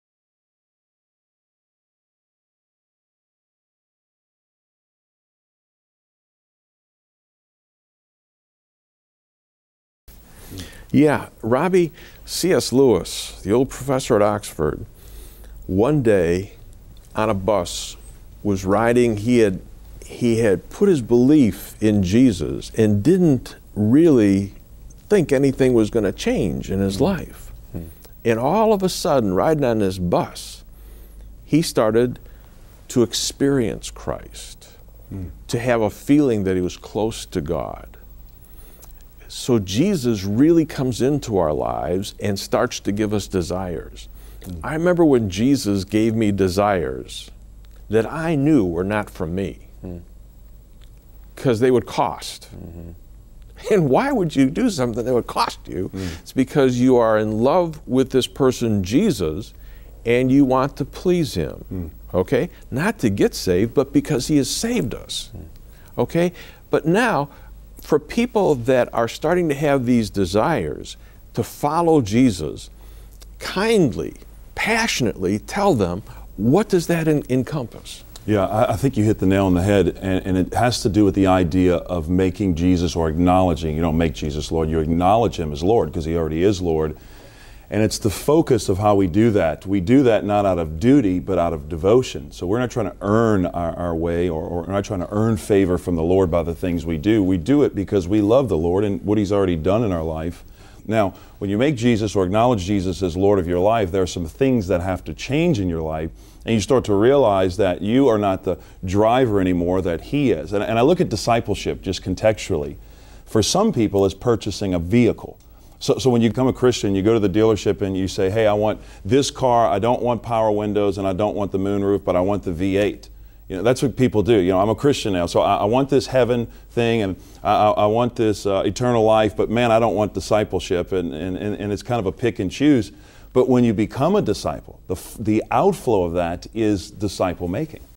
What does “following Christ” really look like?
From the series "Are you a Disciple of Jesus?" featuring special guests Robby Gallaty, David Platt, and Bill Hull.